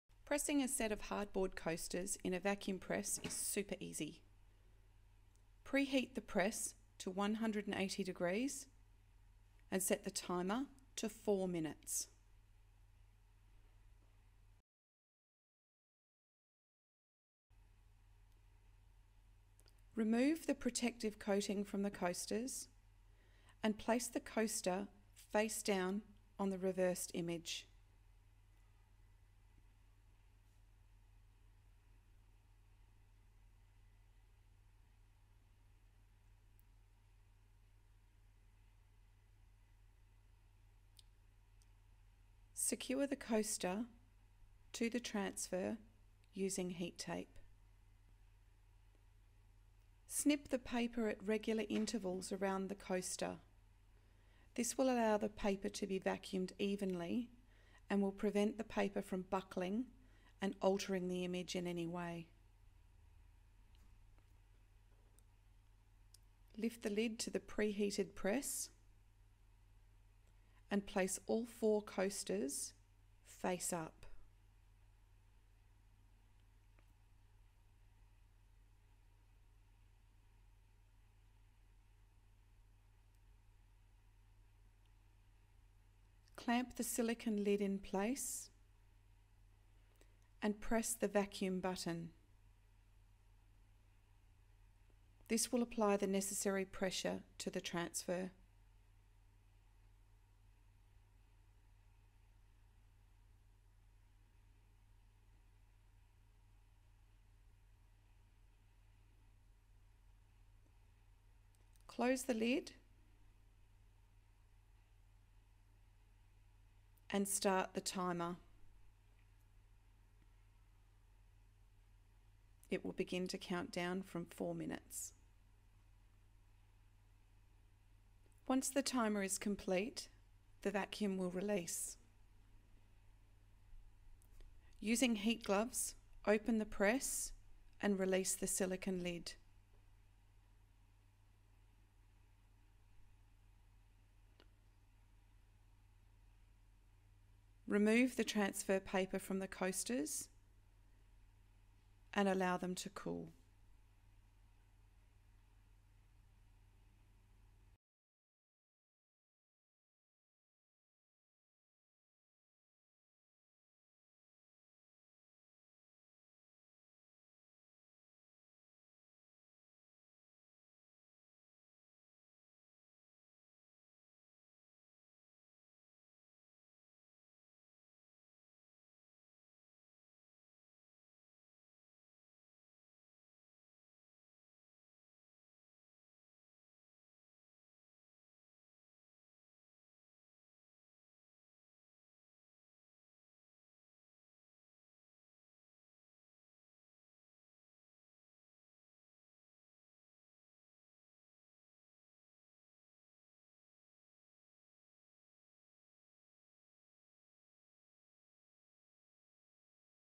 How to use a 3D Vacuum Heat Press to sublimate hardboard coasters.
In this 'how to' video, we demonstrate how to press a set of 4 hardboard coasters in a 3D Vacuum Heat Press (available at Ceramicraft).